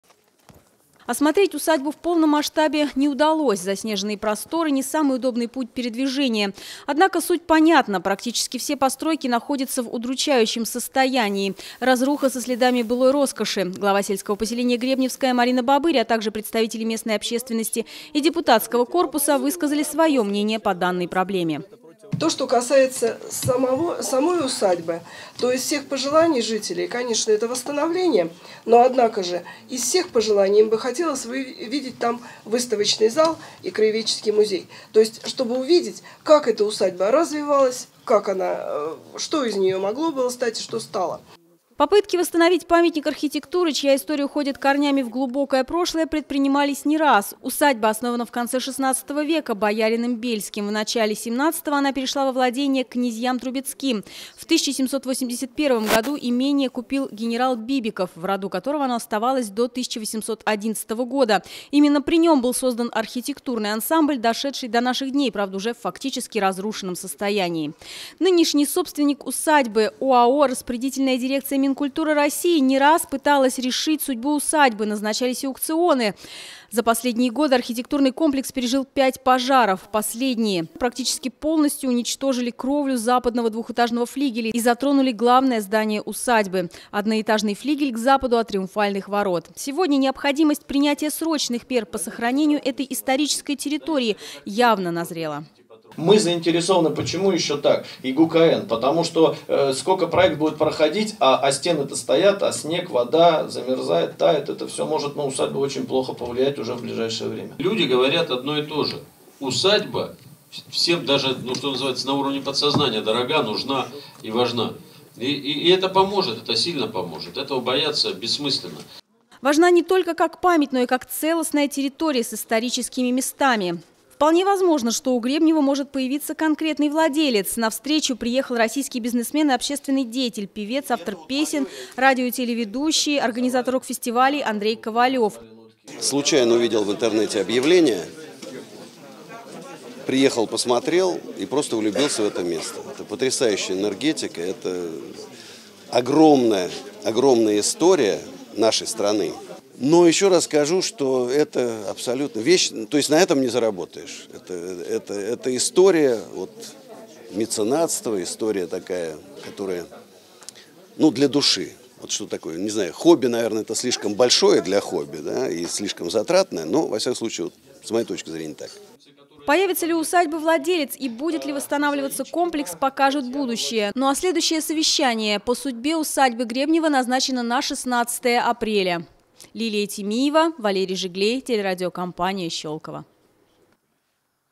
Что будет с усадьбой Гребнево?
Что делать с тем, что осталось от некогда роскошного комплекса- усадьбы Гребнево? Руины и безнадзорность сегодня- это фактическое состояние усадьбы. Решить судьбу и определить, что же делать с историческим местом в Гребневском поселении, приехала специальная  комиссия, в составе которой- представители Министерства культуры России и Московской области.